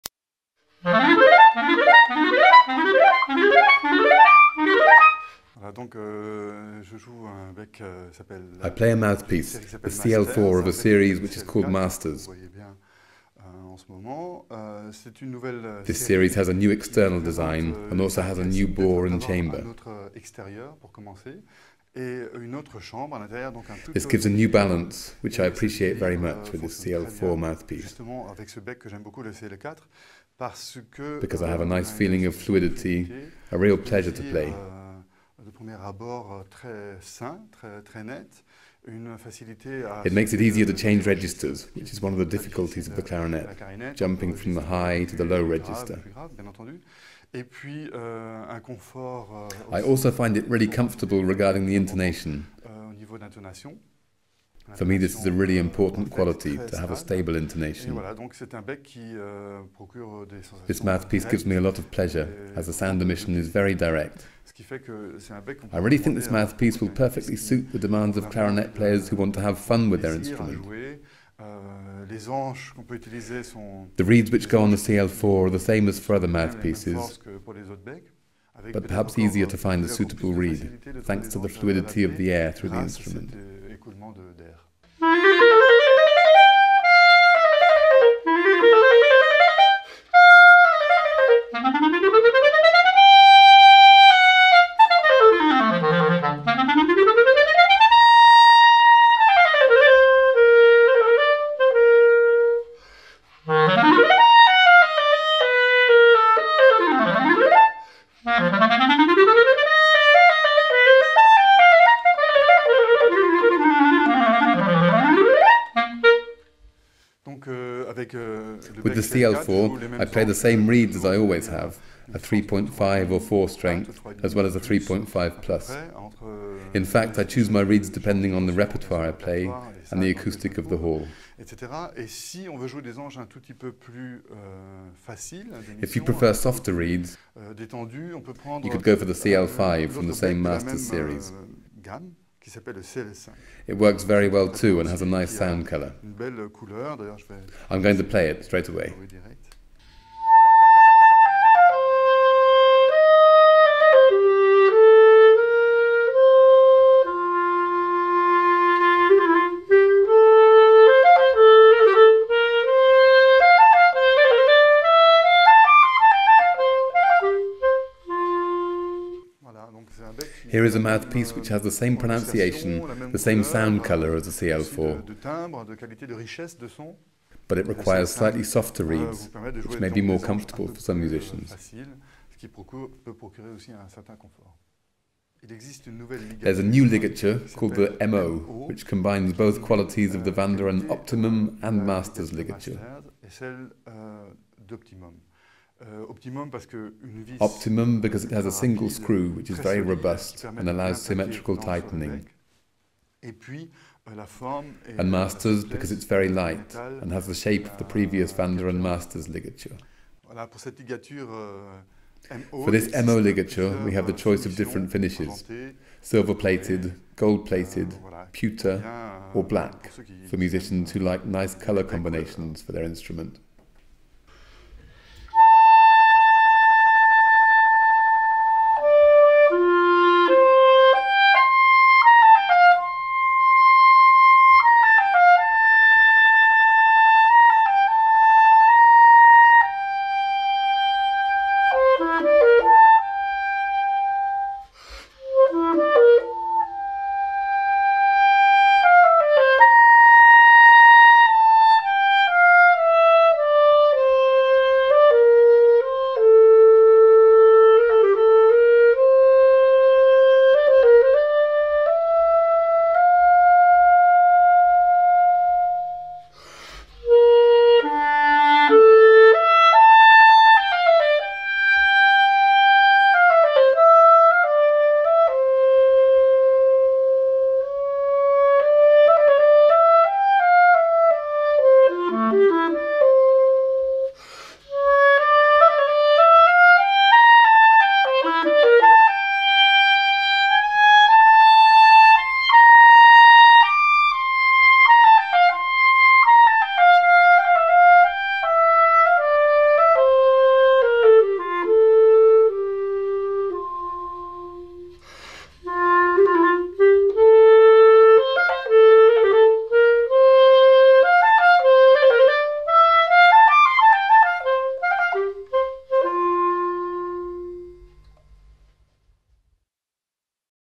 Vandoren USA_ Paul Meyer on the new Masters Mouthpiece.mp4
Listen as clarinet legend Paul Meyer talks about and demonstrates the new Masters mouthpiece from Vandoren..